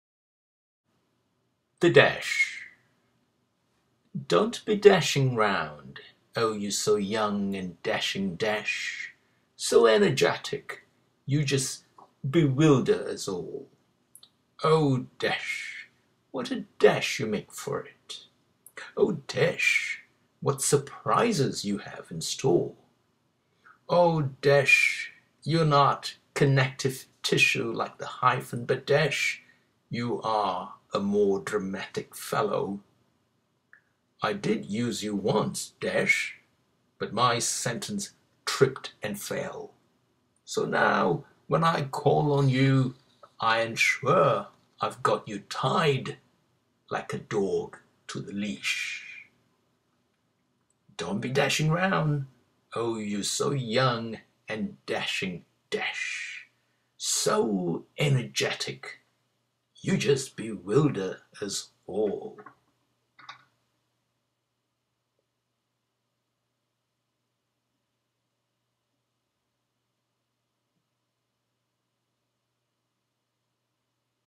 The Dash
The Dash - PUNCTUATION POEMS (4 of 12)

don't be dashing round
oh you so young and dashing dash;
so energetic --
you just bewilder us all

O dash --
what a dash you make for it;
O dash --
what surprises you have in store

O dash --
you're not connective tissue
like the hyphen;
but dash -
you are a more dramatic fellow

I did use you once, dash -
but my sentence tripped and fell;
so now when I call on you
I ensure I've got you tied --
like a dog to the leash